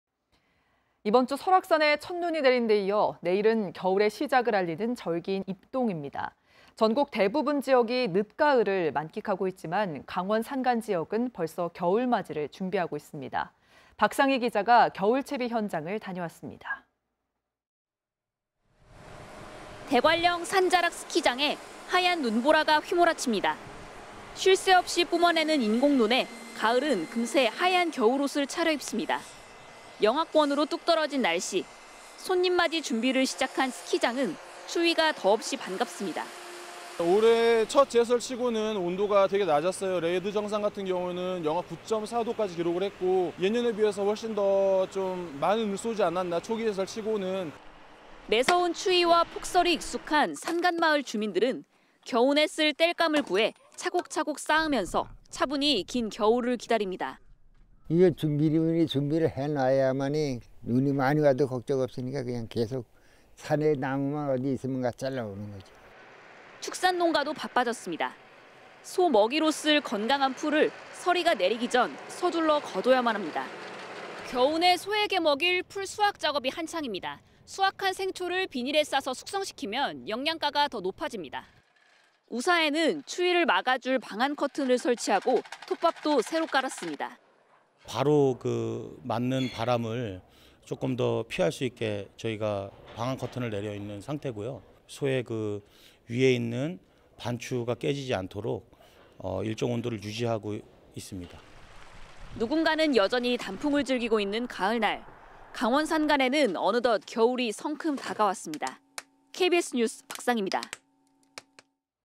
내일 ‘입동’…강원 산간은 벌써 겨울 채비
[앵커]

 이번 주 설악산에  첫눈이 내린 데 이어, 내일(7일)은 겨울의 시작을 알리는 절기인 '입동'입니다.

 전국 대부분 지역이 늦가을을 만끽하고 있지만, 강원 산간지역은  벌써 겨울맞이를 준비하고 있습니다.

 박상희 기자가 겨울 채비 현장을 다녀왔습니다.

 [리포트]

 대관령 산자락 스키장에 하얀 눈보라가 휘몰아칩니다.

 쉴 새 없이 뿜어내는 인공눈에 가을은 금세 하얀 겨울옷을 차려입습니다.

 영하권으로 뚝 떨어진 날씨.

 손님맞이 준비를 시작한 스키장은 추위가 더없이 반갑습니다.

 [정현배/용평리조트 레저지원팀 : "올해 첫 제설치고는 온도가 되게 낮았어요. 레드(상급자 코스) 정상 같은 경우는 영하 9.4도까지 기록을 했고. 예년보다 훨씬 더 많은 눈을 쏘지 않았나, 초기 제설치고는."]

 매서운 추위와 폭설이 익숙한 산간 마을 주민들은, 겨우내 쓸 땔감을 구해 차곡차곡 쌓으면서 차분히 긴 겨울을 기다립니다.

 [박승원/강릉시 왕산면 : "이게 준비를, 미리미리 준비해놓아야만 눈이 많이 와도 걱정 없으니까 그냥 계속 산에 나무만 어디 있으면 가서 잘라오는 거죠."]

 축산 농가도 바빠졌습니다.

 소먹이로 쓸 건강한 풀을 서리가 내리기 전 서둘러 거둬야만 합니다.

 겨우내 소에게 먹일 풀 수확 작업이 한창입니다.

 수확한 생초를 비닐에 싸서 숙성시키면, 영양가가 더 높아집니다.

 우사에는 추위를 막아줄 방한 커튼을 설치하고, 톱밥도 새로 깔았습니다.

 [박용현/축산 농민 : "바로 맞는 바람을 조금 더 피할 수 있게 저희가 방한 커튼을 내려놓은 상태고요. 소의 위에 있는 반추(되새김질)가 깨지지 않도록 일정 온도를 유지하고 있습니다."]

 누군가는 여전히 단풍을 즐기고 있는 가을날, 강원 산간에는 어느덧 겨울이 성큼 다가왔습니다.

 KBS 뉴스 박상희입니다.

 촬영기자:김남범